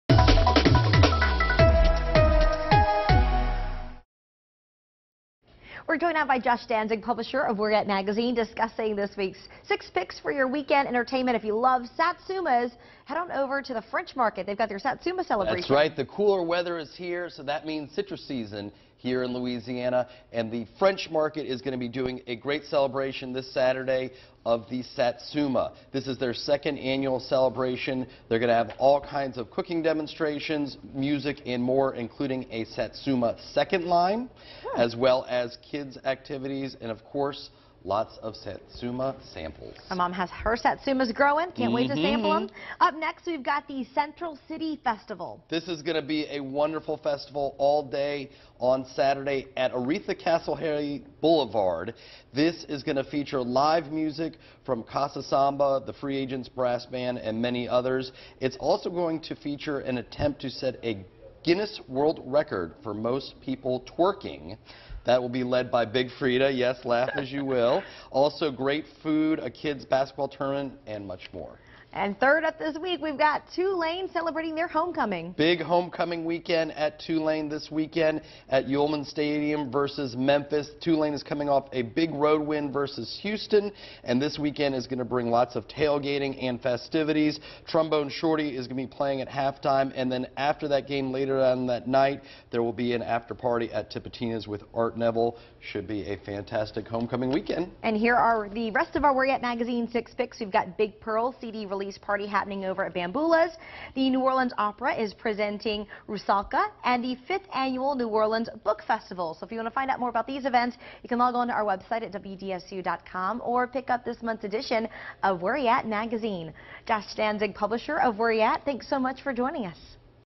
Where Y'at Magazine's 6 picks for weekend of Nov. 15, 16
WDSU speaks with Josh Danzig from Where Y'at Magazine to get your six picks for your weekend entertainment.